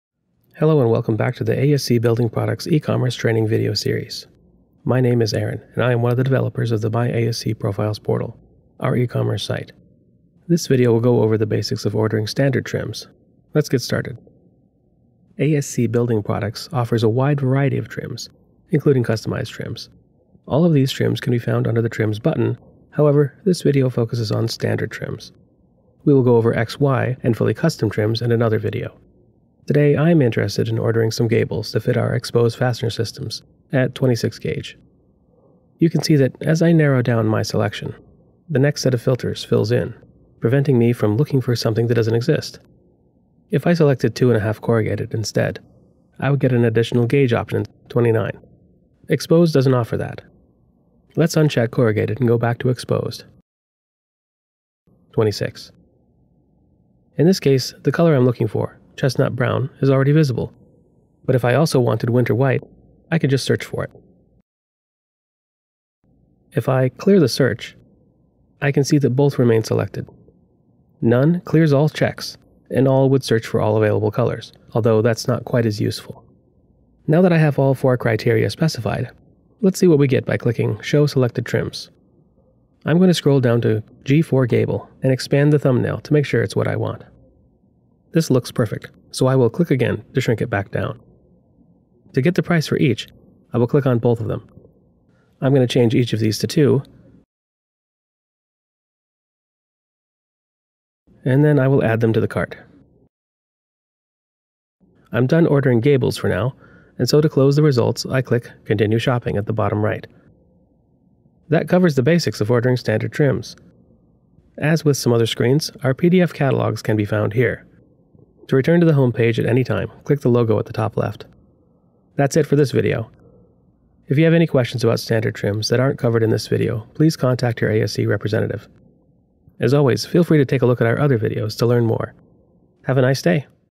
Metal Roofing Ordering through E-Commerce. MyASCProfiles Video #4: Standard Trims
Ordering metal roofing through our E-Commerce. Description Video Training Series. Customers ONLY